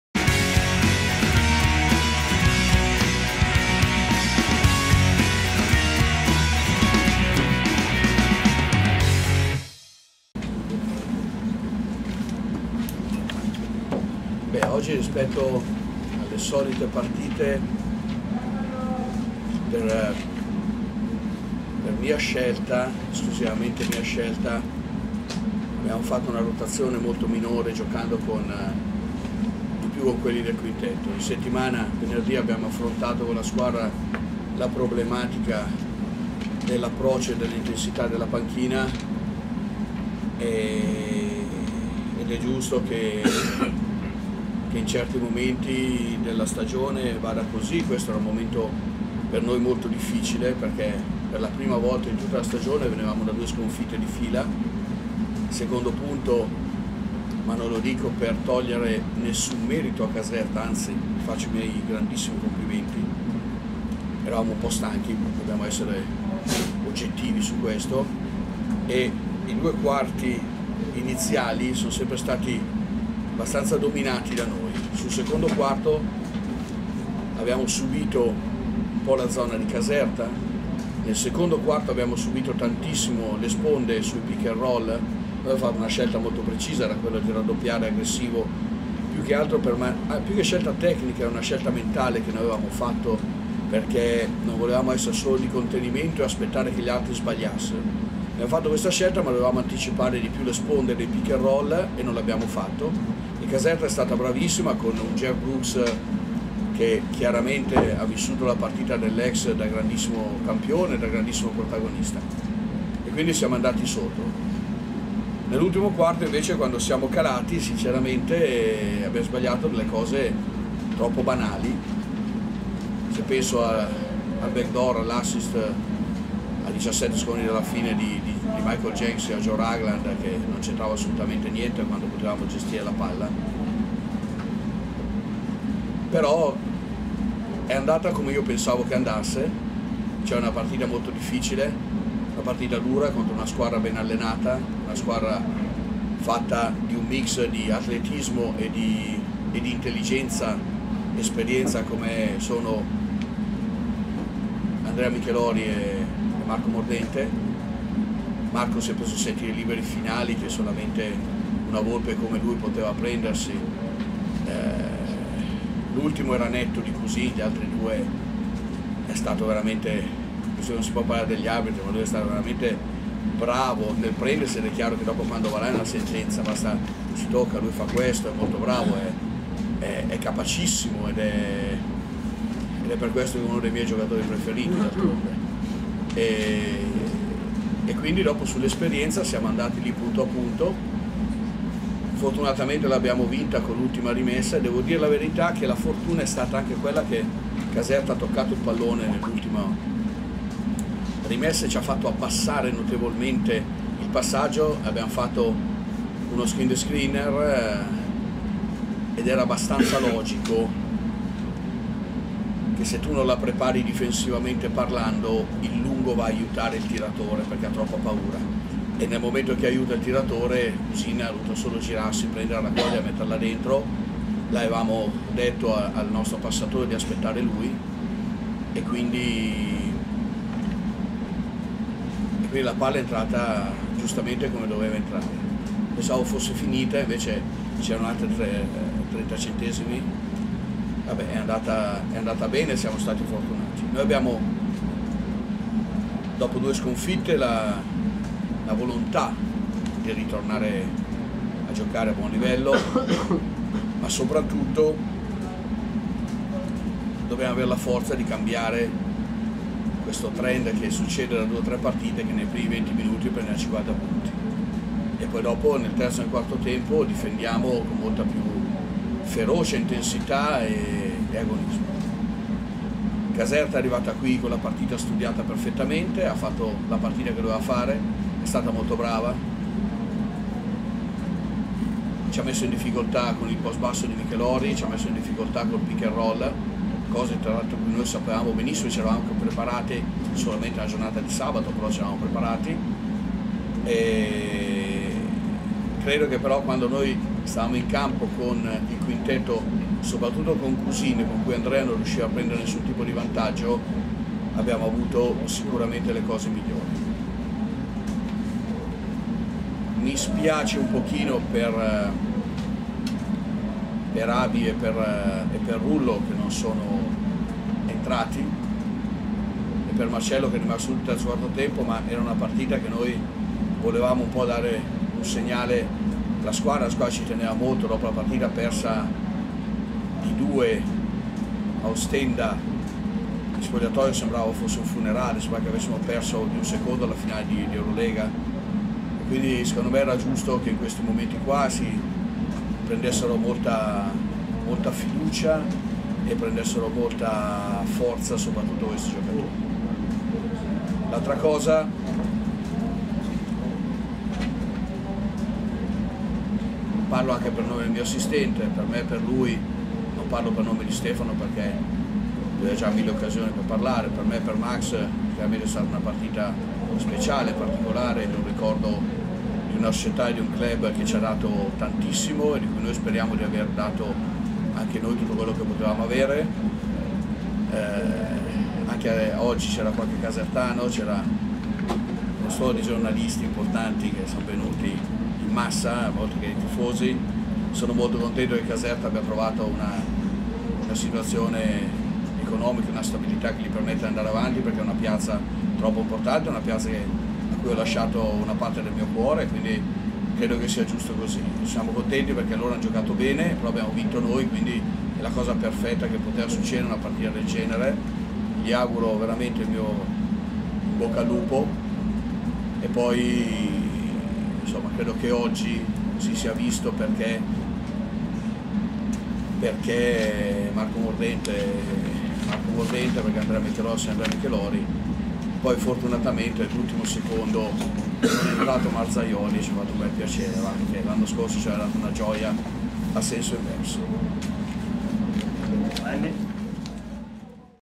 Stefano Sacripanti dopo Acqua Vitasnella Cantù Pasta Reggia Caserta
coach Stefano Sacripanti dopo Acqua Vitasnella Cantù - Pasta Reggia Caserta